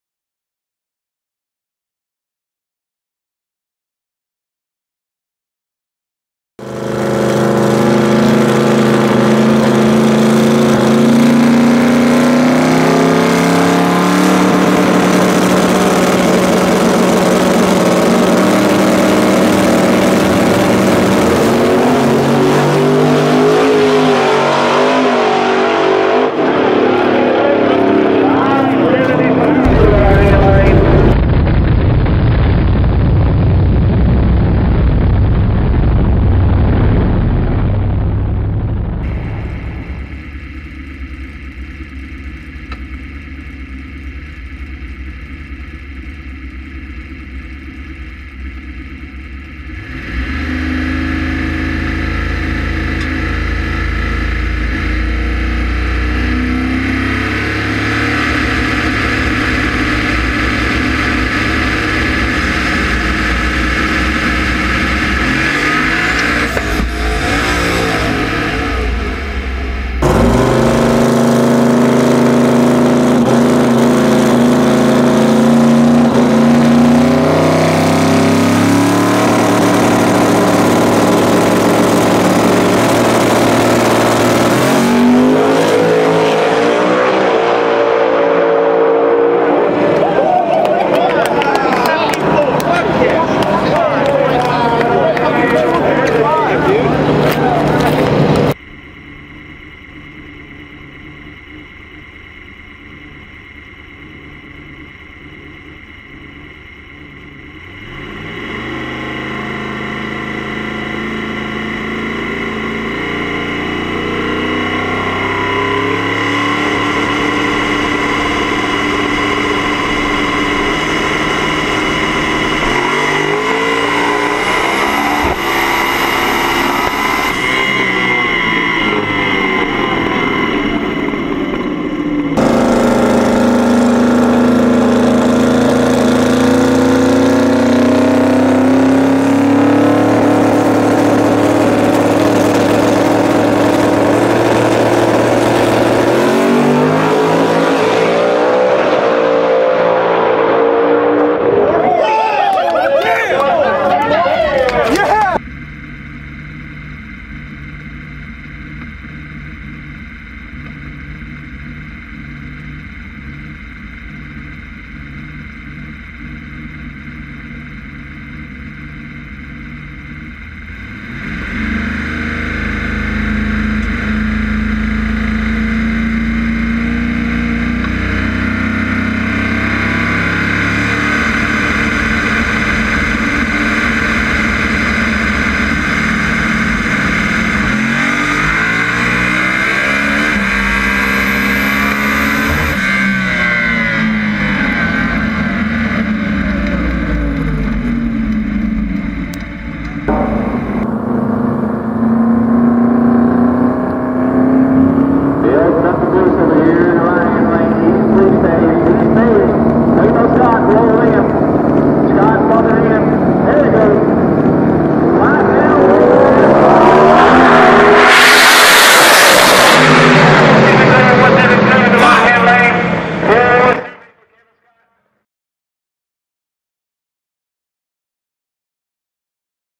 LMR Fall Brawl 2013 Compilation
A Compilation of all passes we have video of from the 2013 Fall Brawl race in holly springs.